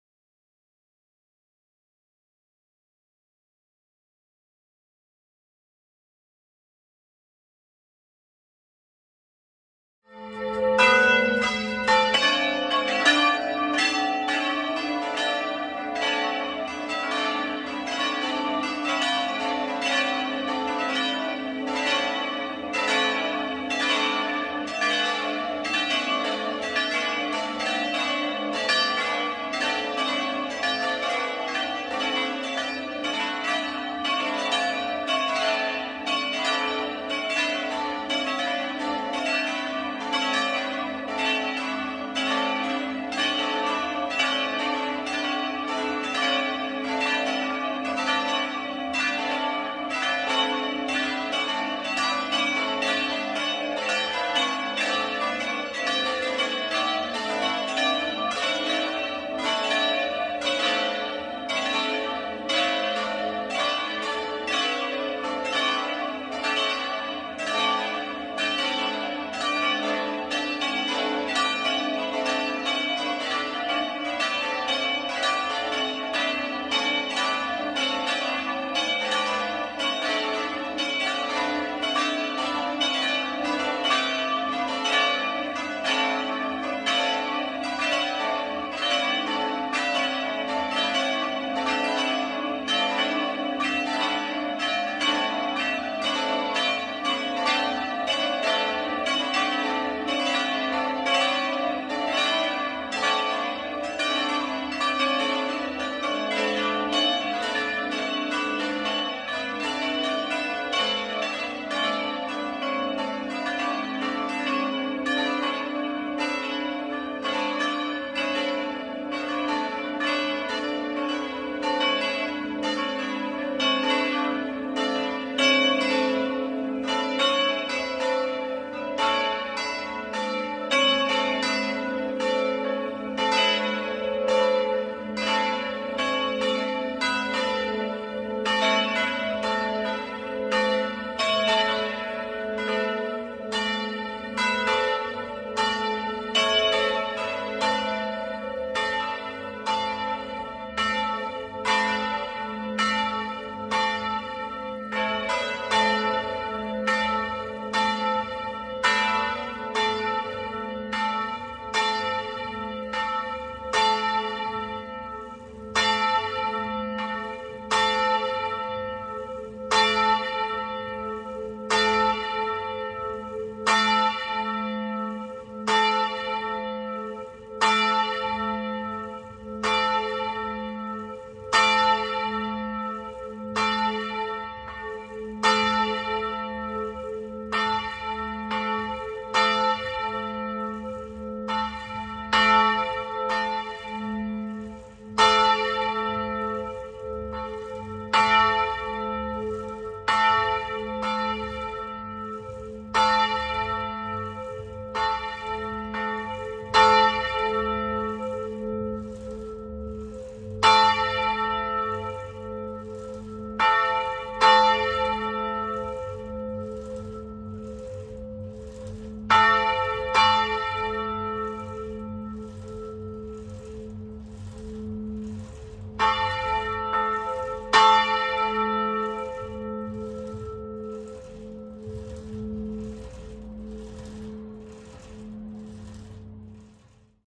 Suonata nella cella campanaria - Campane della Chiesa Parrocchiale dell'Immacolata a Grumo (TN)
Concerto di 5 campane in LA3 a slancio.

Le campane sono state fuse:
- Prima, seconda, terza e quinta da Cesare Brighenti a Bologna nel 1922
- Quarta da Daciano Colbachini a Padova nel 1969

Distesa a Plenum (LA3, SI3, DO#4, RE4, MI4) 30 minuti prima della S. Messa in onore di Sant'Anna. La stessa suonata è stata fatta anche 15 e 2 minuti prima della S. Messa.

VIDEO e AUDIO:
-Iphone 13